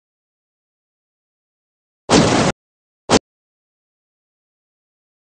non si ferma allo stop, ma...
afferma essersi trattato di inesperienza di guida con la macchina nuova usando contemporaneamente freno ed acceleratore in auto con cambio automatico